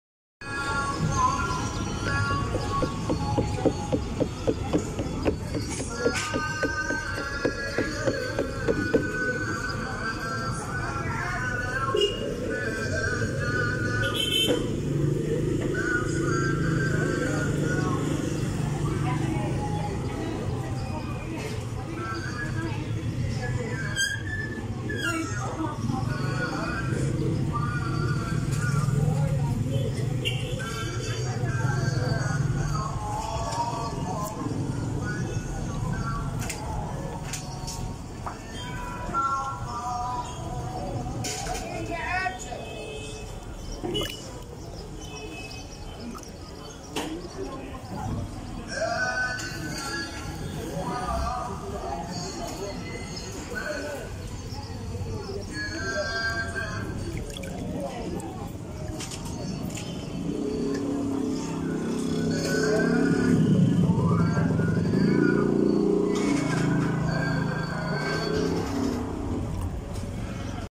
سبحان الله سمكه بلطي كبيره ولسا فيها الروح لحد دلوقت
أكبر سمكه بلطي لسا صاحيه
سمكه بلطي كبيره ولسا صاحيه لحد الان
سبحان الله